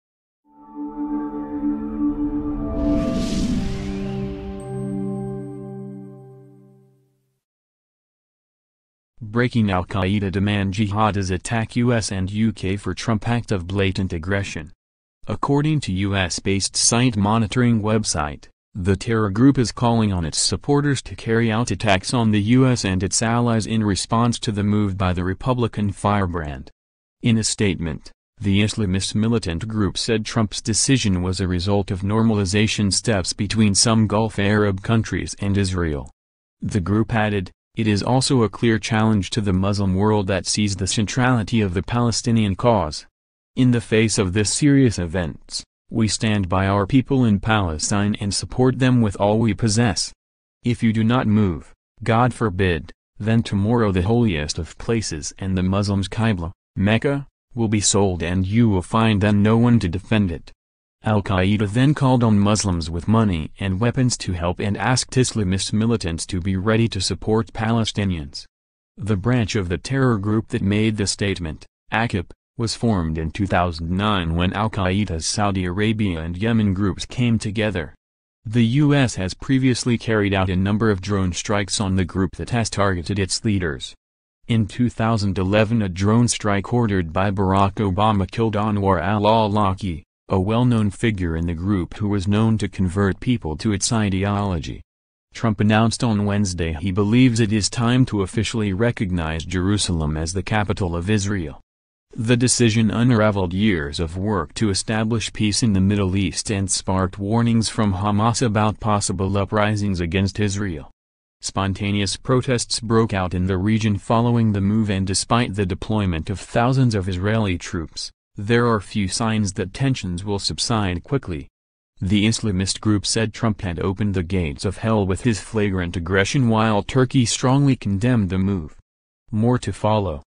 BREAKING Al Qaeda demand jihadis ATTACK US and UK for Trump act of ‘BLATANT AGGRESSION’
According to US-based SITE monitoring website, the terror group is calling on its supporters to carry out attacks on the US and its allies in response to the move by the Republican firebrand.

In a statement, the Islamist militant group said Trump’s decision was a result of “normalisation steps” between some Gulf Arab countries and Israel.

The group added: “It is also a clear challenge to the Muslim world that sees the centrality of the Palestinian cause.

“In the face of this serious events, we stand by our people in Palestine and support them with all we possess.